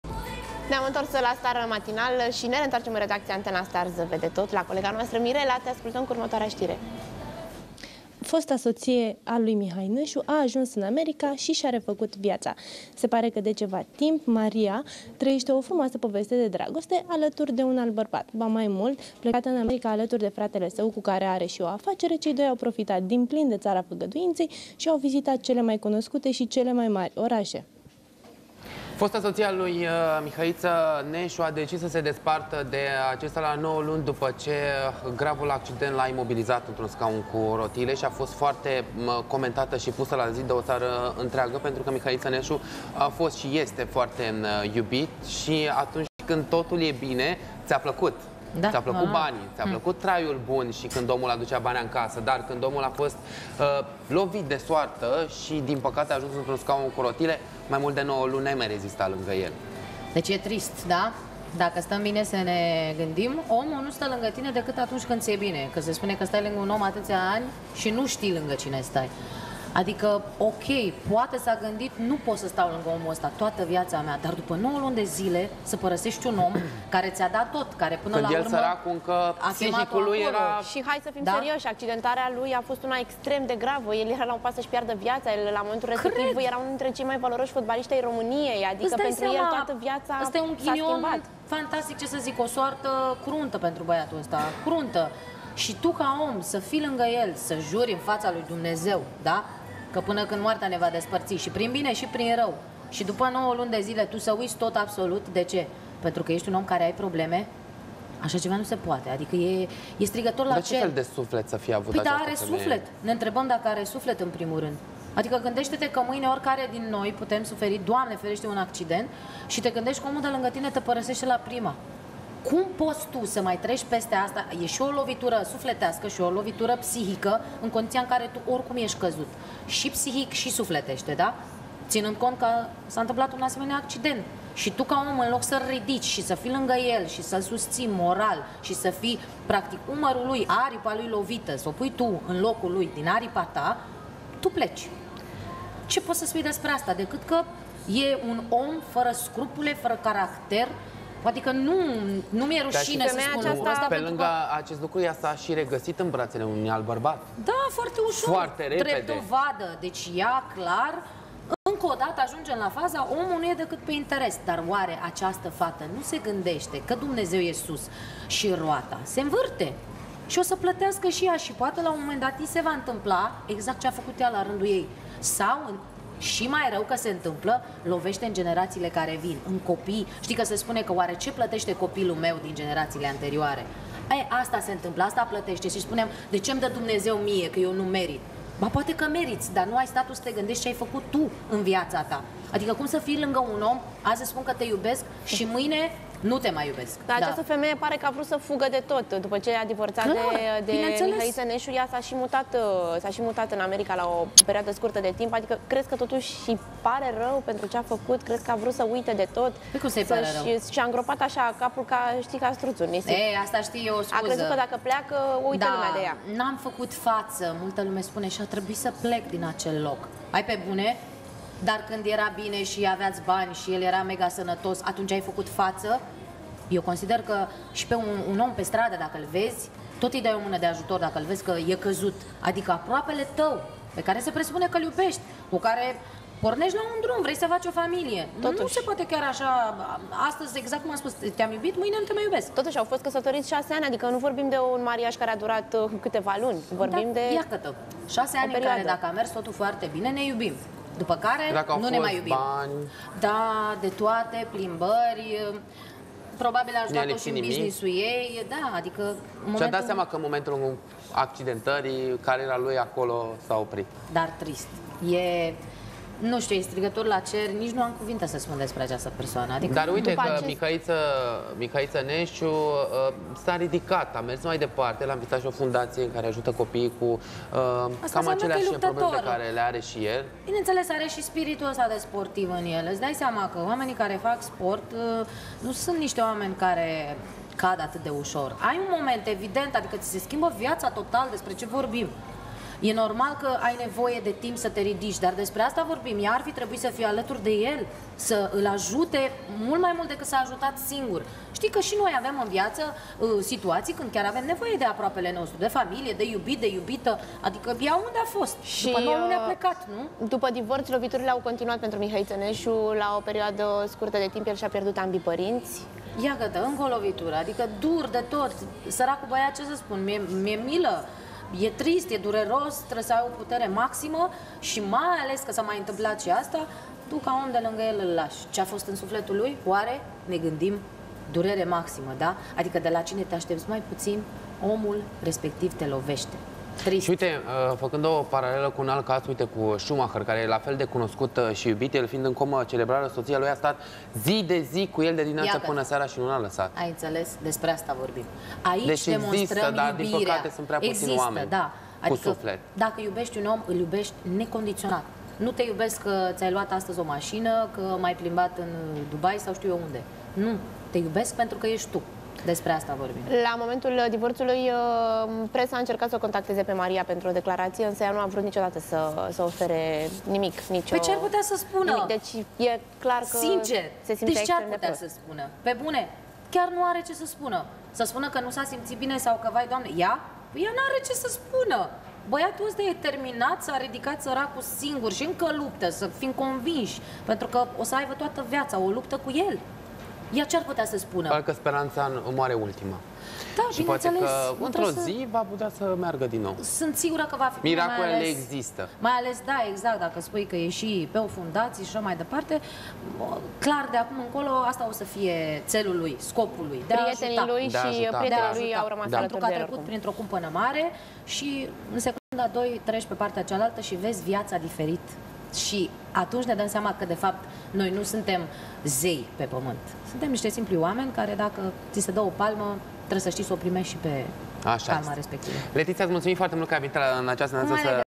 Soția lui Mihai Neșu are un nou iubit. S-a lansat în afaceri în America
Fosta soție a lui Mihai Neșu a ajuns în America și și-a refăcut viața. Se pare că de ceva timp, Maria trăiește o frumoasă poveste de dragoste alături de un alt bărbat.